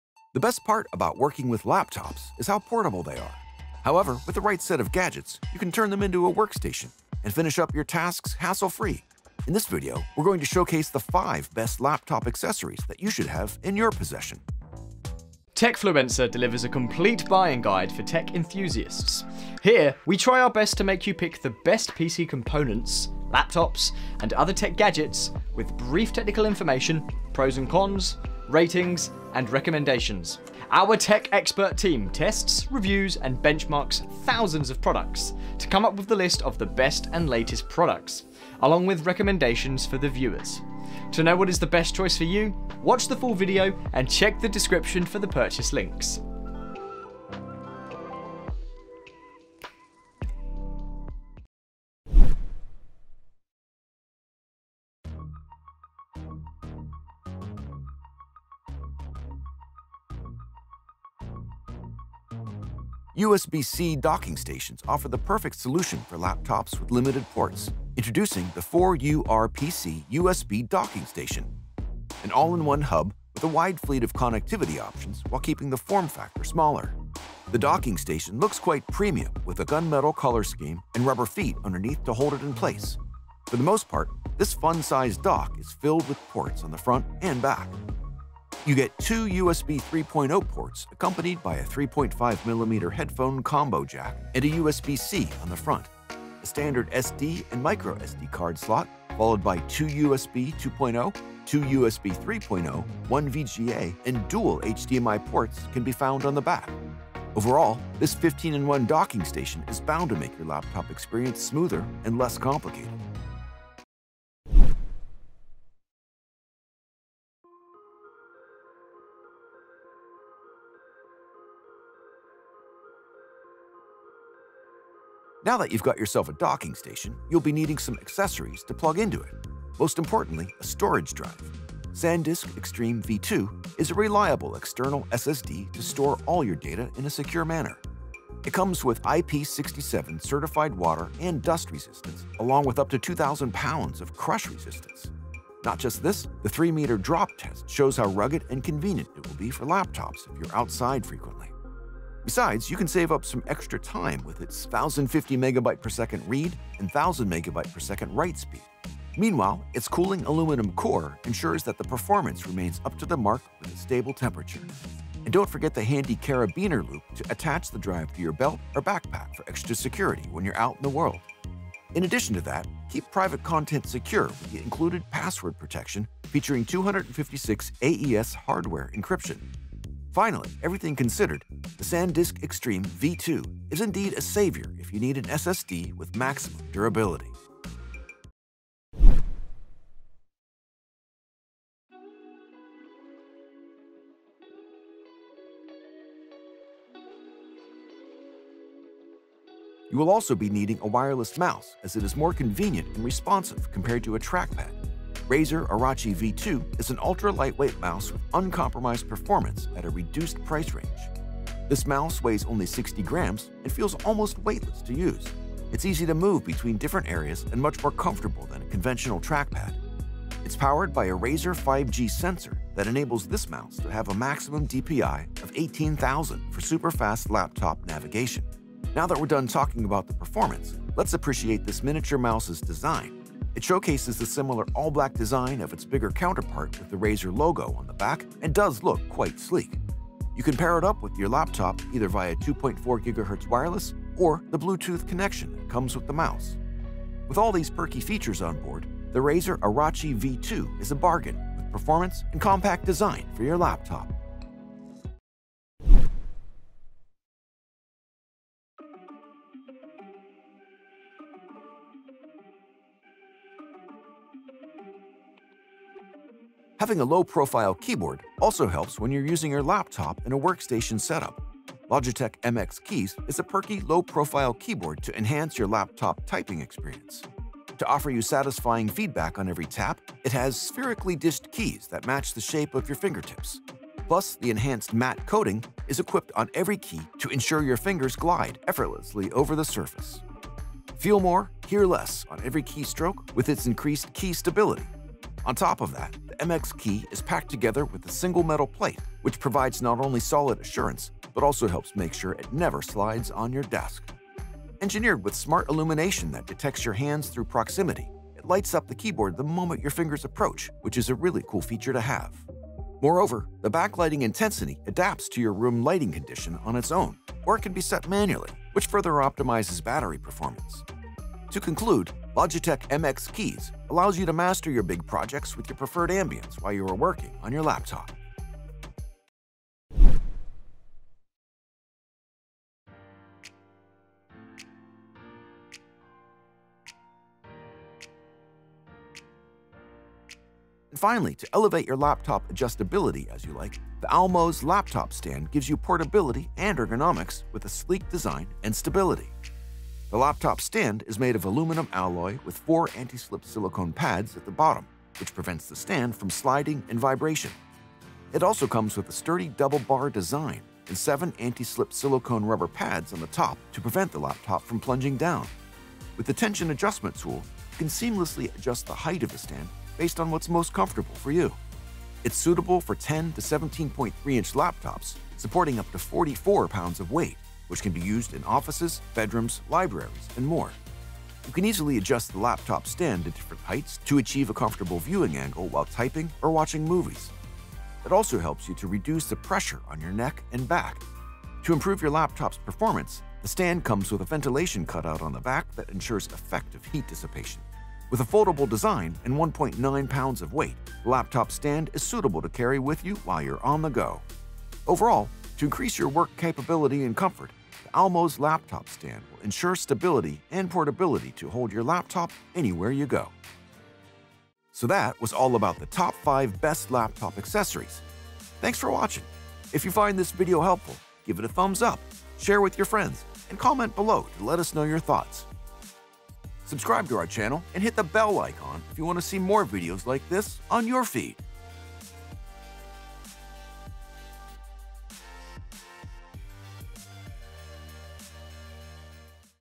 5 Coolest PC Accessories That Are Worth Using ▶ 2
Coolest PC Accessories can elevate how you work with your PC and enhance your productivity smoothly. These PC gadgets come in all shapes and sizes and offer limitless possibilities of what you can do with your setup. So here are the 5 Coolest PC Accessories That Are Worth Using.

0:00 - Introduction

00:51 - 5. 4URPC - USB C Laptop Docking Station

01:53 - 4. SanDisk Extreme V2

03:08 - 3. Razer Orochi V2

04:15 - 2. Logitech MX Keys

05:29 - 1. Almoz Laptop Stand (Sponsor)